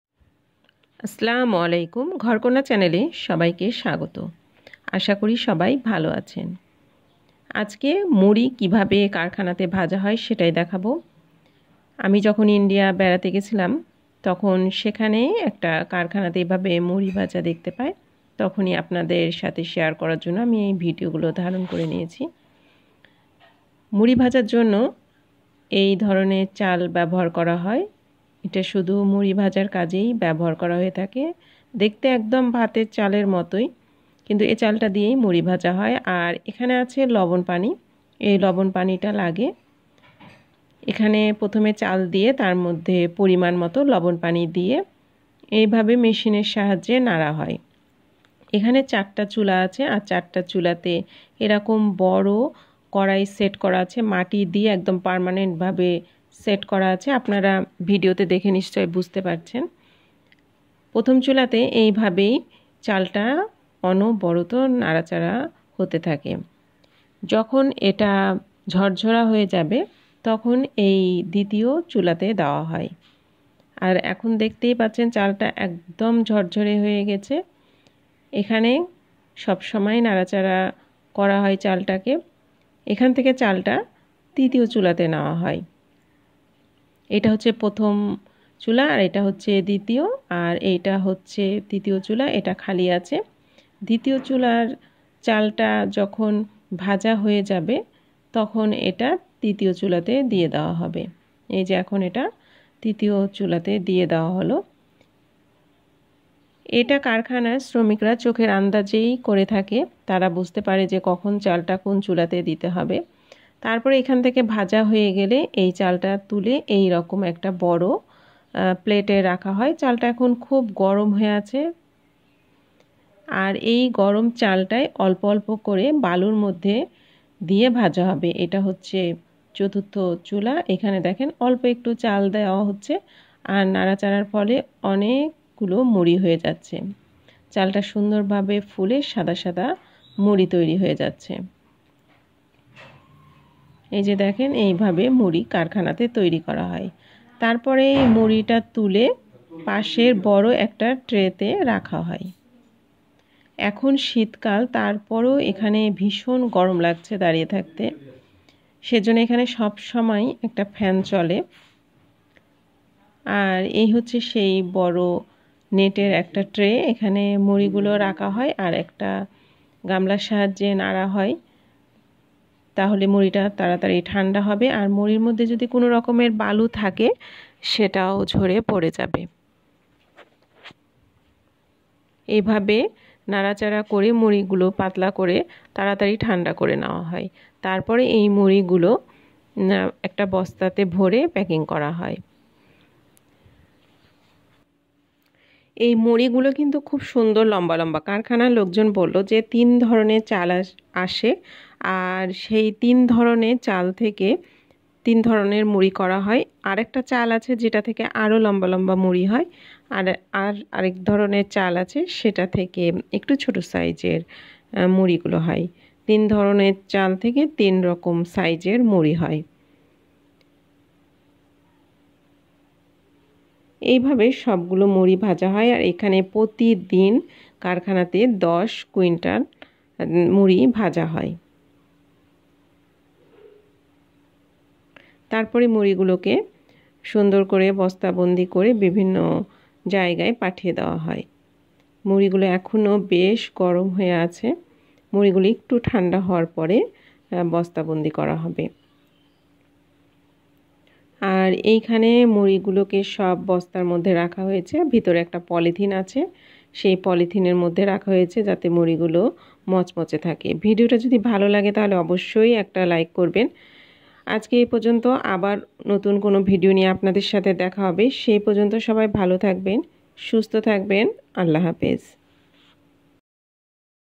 How Puffed Rice Are Made In The Factory. মুড়ি তৈরি।
Assalamu Alaikum. welcome to GHAR KONYA channel.
Remember to click on the bell button to get the latest video of GHAR KONYA !
Allah Hafiz.